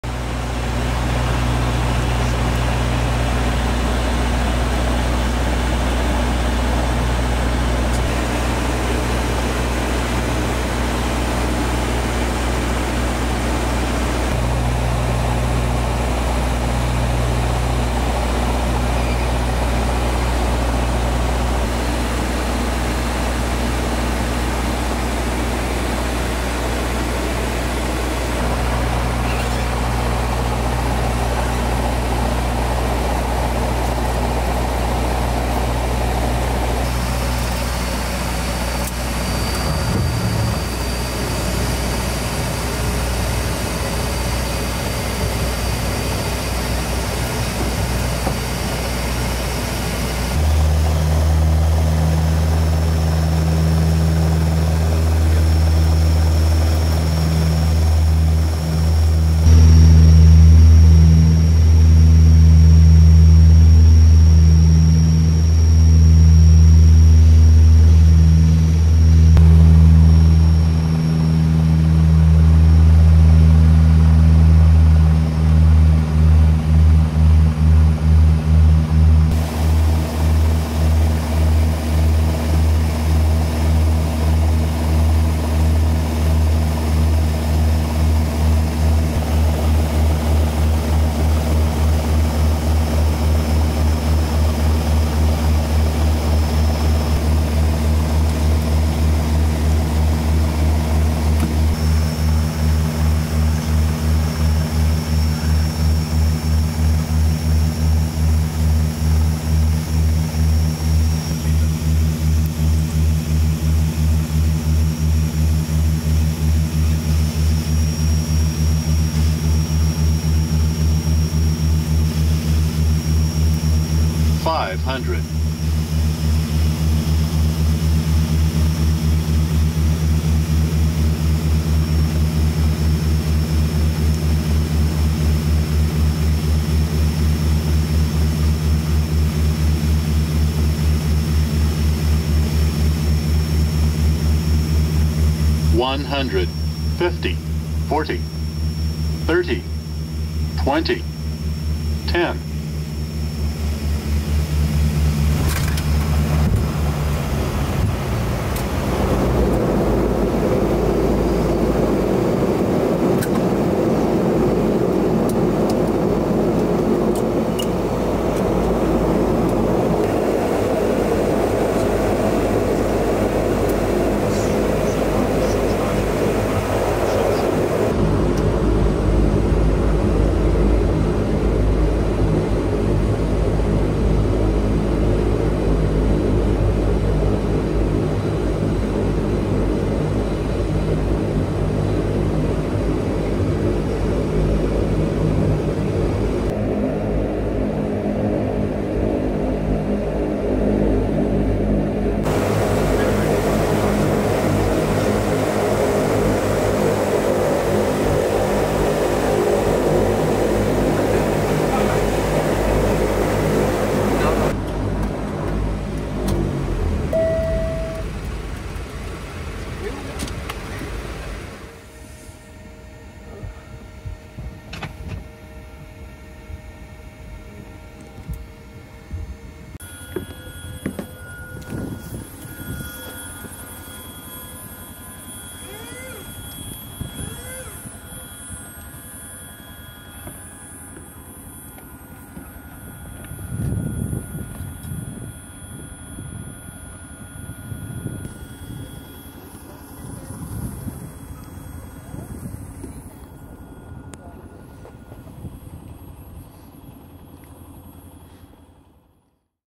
Landing in Nuuk, Greenland - Air Greenland Dash 8-200 | COCKPIT VIEW
Air Greenland
De Havilland Canada Dash 8-200 (OY-GRP)
Flight Number: GL711
Route: KEF to GOH
Date of Travel: March 29, 2023

Nuuk Airport
Nuuk, Greenland